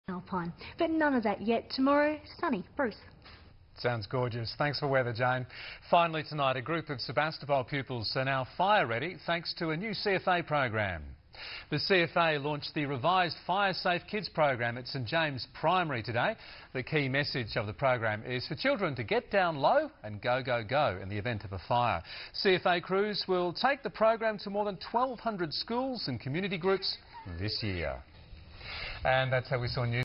Fire Safe Kids launch - WIN TV
Children at St James Parish School learnt to \"get down low and go go go\" this week as part of CFA\'s renewed school education program -Fire Safe Kids